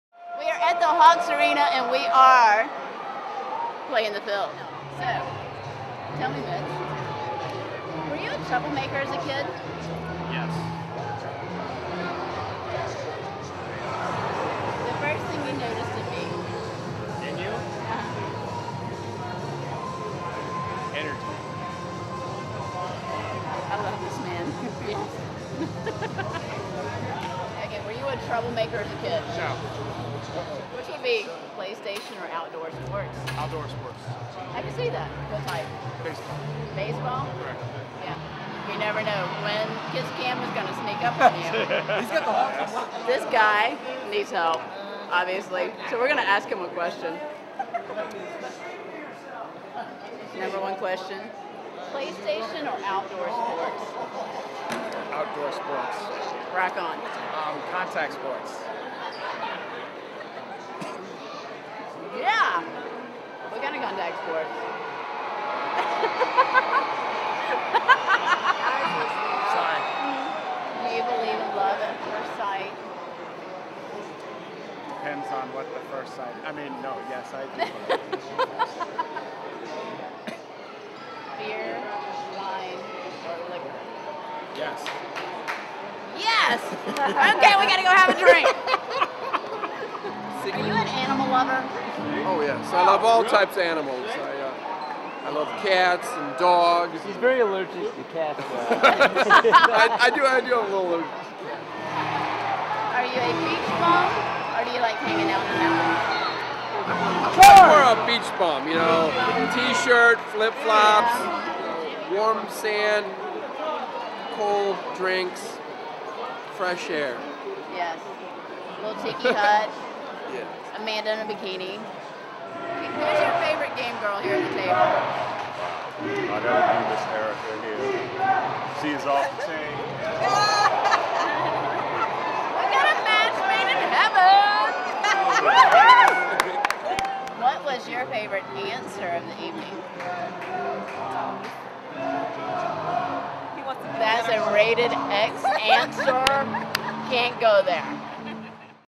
What's Your Answer?
Find out how the guys answered the question! Amanda had the kiss cam crew on the move getting the down low from the quys on how they would answer questions from her game, Playing the Field.